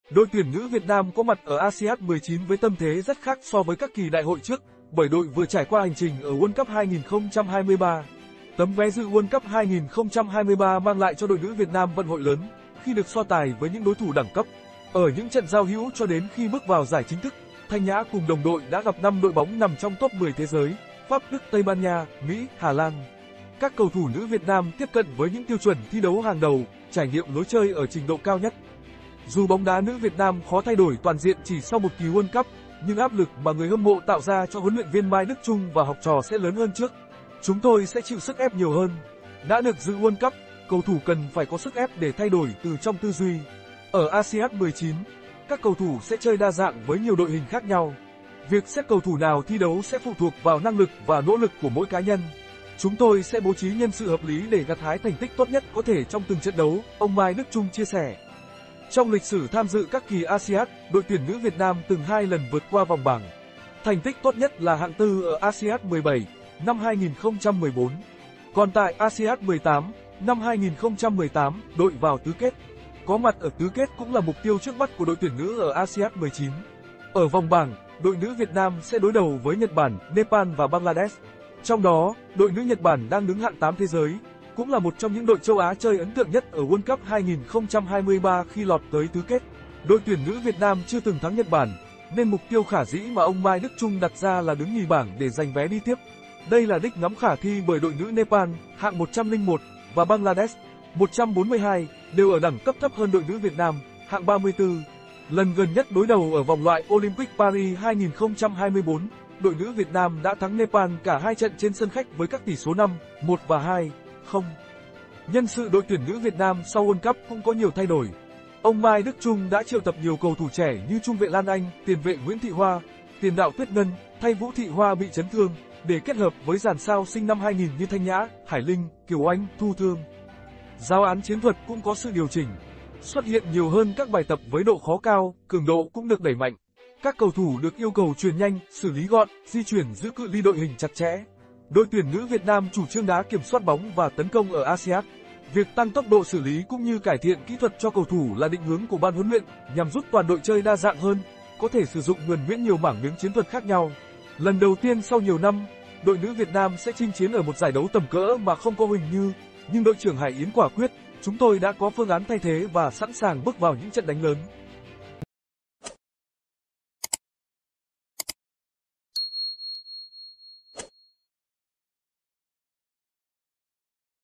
ĐỘI TUYỂN BÓNG ĐÁ NỮ VIỆT NAM CHINH PHỤC ASIAD 19 MÀ KHÔNG CÓ HUỲNH NHƯ
Đội tuyển nữ Việt Nam có mặt ở ASIAD 19 với tâm thế rất khác so với các kỳ đại hội trước, bởi đội vừa trải qua hành trình ở World Cup 2023. Tấm vé dự World Cup 2023 mang lại cho đội nữ Việt Nam vận hội lớn, khi được so tài với những đối thủ đẳng cấp. Ở những trận giao hữu cho đến khi bước vào giải chính thức, Thanh Nhã cùng đồng đội đã gặp 5 đội bóng nằm trong tốp 10 thế giới: Pháp, Đức, Tây Ban Nha, Mỹ, Hà Lan. Các cầu thủ nữ VN tiếp cận với những tiêu chuẩn thi đấu hàng đầu, trải nghiệm lối chơi ở trình độ cao nhất.